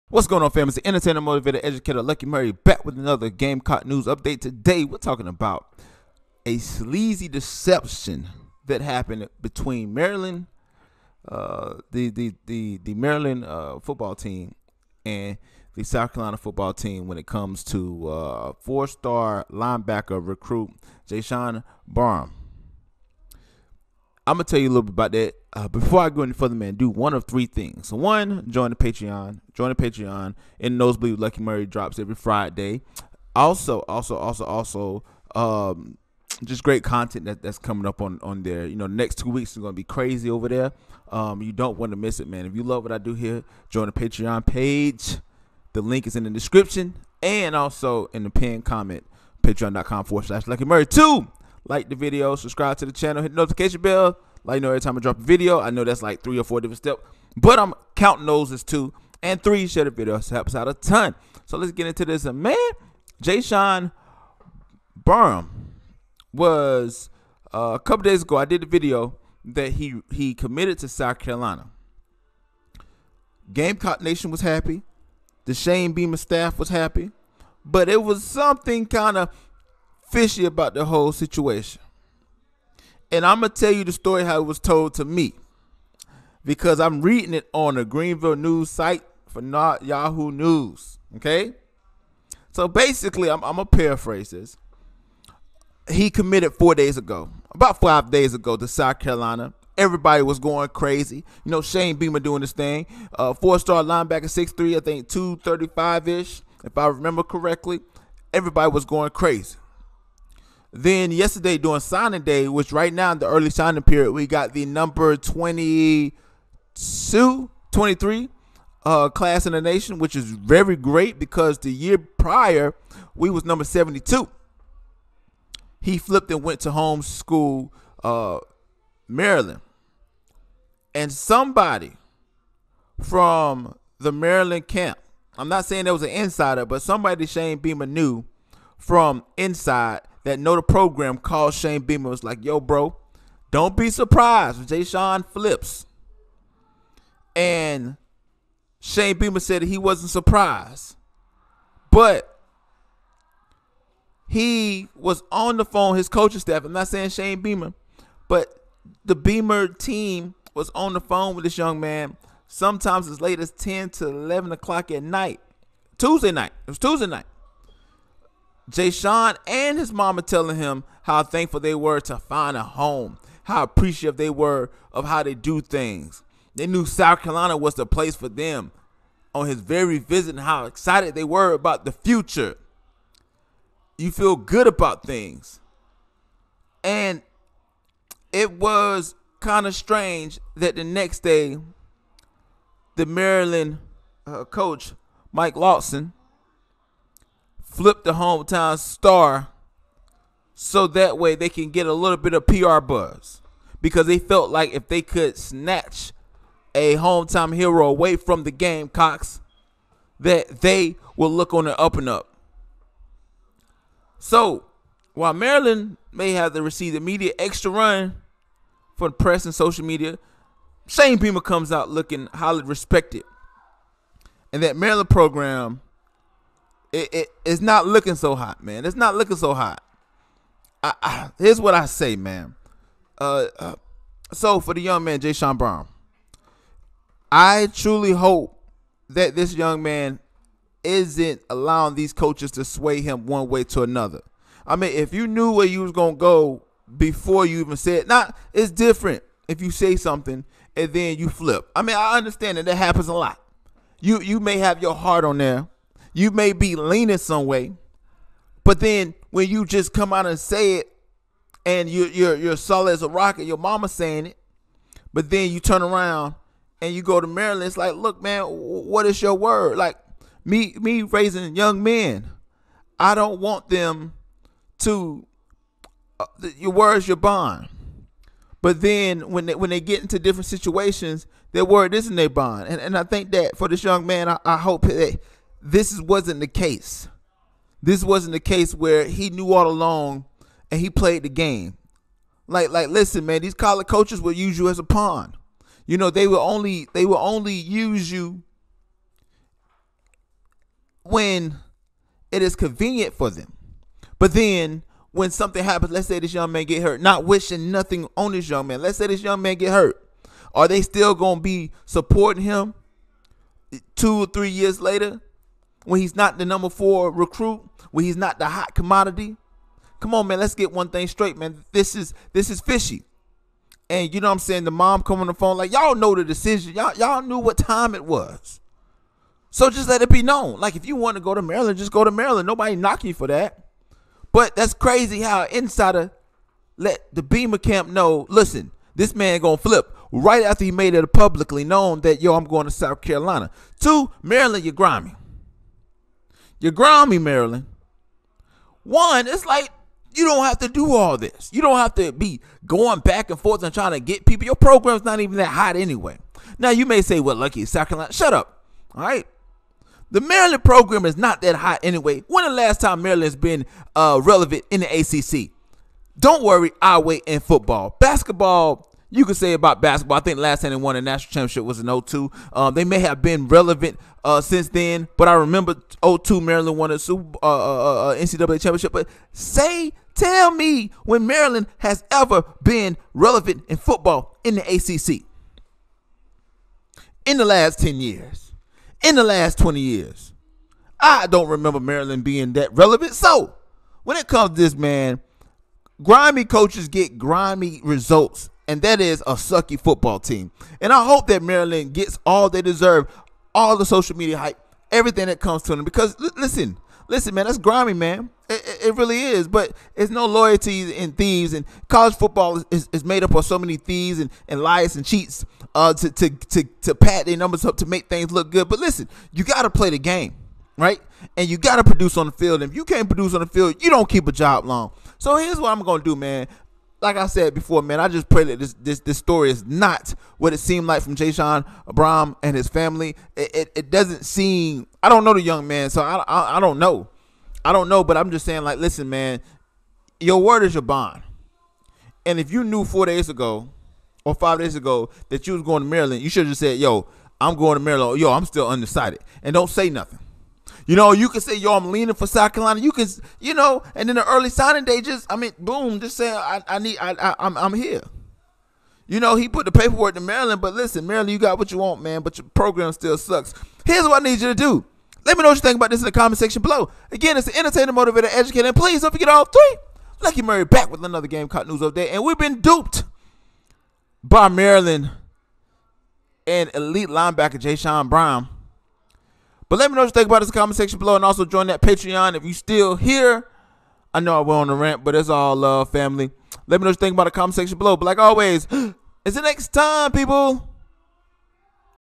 Maryland Terrapins STOLE TOP PROSPECT Jaishawn Barsham From South Carolina Gamecocks
Recruits flipping on signing day from one Power Five school to another always causes quite a stir, but the last minute about face an elite linebacker pulled on South Carolina Wednesday was a doozy.

As it turns out that was the whole rather sleazy point coming from Maryland's view after hearing USC coach Shane Beamer describe the situation as nothing more than an underhanded elaborate ruse.

Top prospect Jaishawn Barsham caused quite a celebration within the Gamecocks program Saturday when he publicly announced, clad in USC apparel along with family members that he was committing to USC. It was a huge get for Beamer and his staff with Barsham ranked the No. 108 prospect and eighth-best linebacker in the 247Sports Composite.

He was by far the highest-rated commit of Beamer's two recruiting classes with the next highest signee this year being safety Keenan Nelson Jr. at 219th overall and 19th at his position.